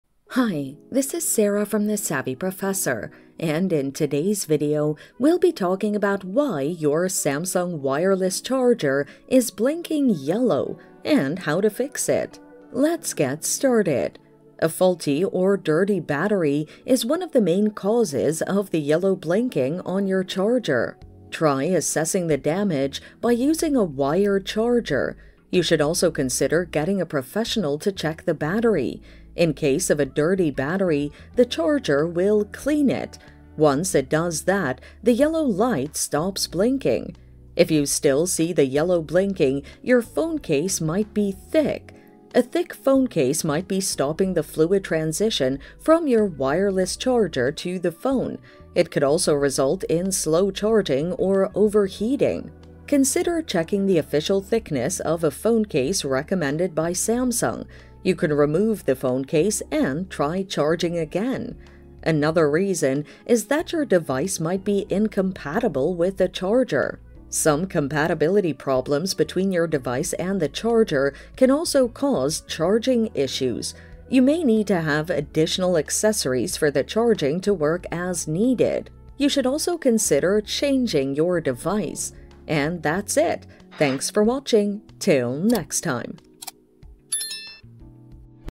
Samsung Wireless Charger Blinking Yellow (What Does Yellow Blinking Indicates? - Best Solutions!)
Samsung Wireless Charger Blinking Yellow (What Does Yellow Blinking Indicates? - Best Solutions!). In this video tutorial I will show you how to fix Samsung wireless charger blinking yellow.

A Faulty or dirty battery is one of the main causes of the yellow blinking on your charger.
Try assessing the damage by using a wired charger. You should also consider getting a professional to check the battery. In the case of a dirty battery, the charger will clean it. Once it does that, the yellow light stops blinking.

If you still see the yellow blinking, your phone case might be thick.
A thick phone case might be stopping the fluid transition from your wireless charger to the phone. It can also result in slow charging or overheating. Consider checking the official thickness of a phone case recommended by Samsung. You can remove the phone case and try charging again.

Another reason is that your device might be incompatible with the charger.
Some compatibility problems between your device and the charger can cause charging issues.  You may need to have additional accessories for the charging to work as needed. You should also consider changing your device.